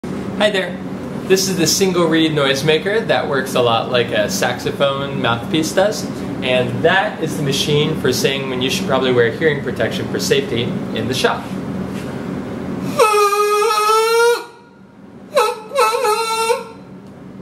The Most Marvelous Mouthpiece
This is the single reed noisemaker made out of PVC. It sounds like a duck and costs nothing to make. Coming to some eardrums near you!